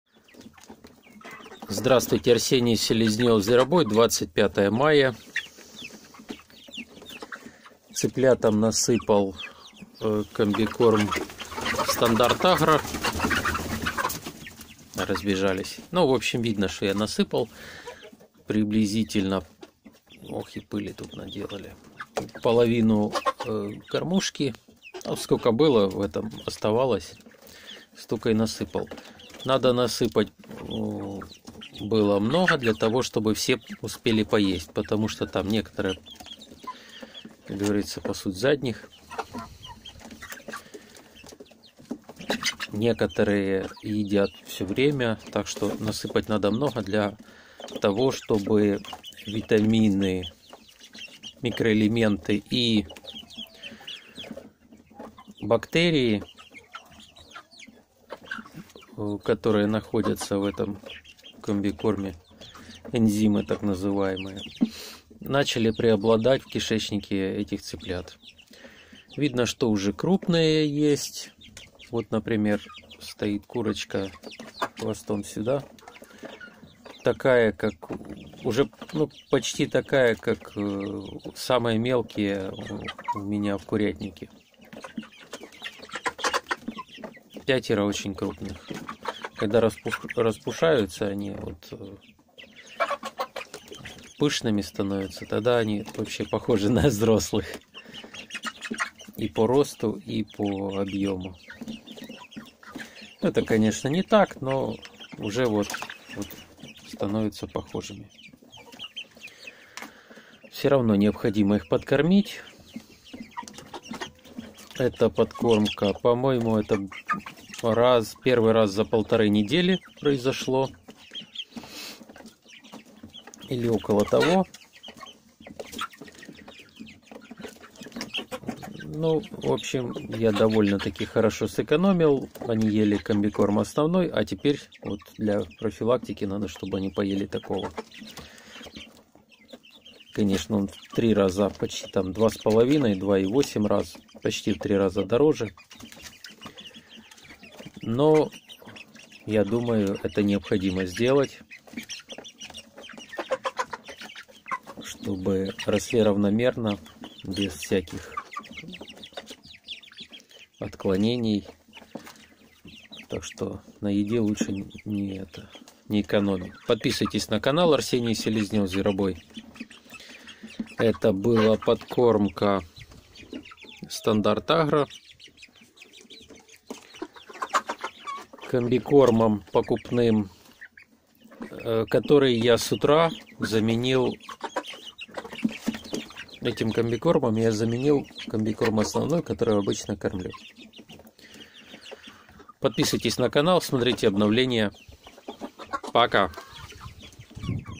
Кормление цыплят специальным комбикормом ради сбалансированного питания
Когда переходите на кормление комбикормом Основной, который в 2,8 раза дешевле покупного, следует помнить о внесении разнообразия. Весна изобилует зеленью, но раз в неделю всё же кормите дополнительно Стартом.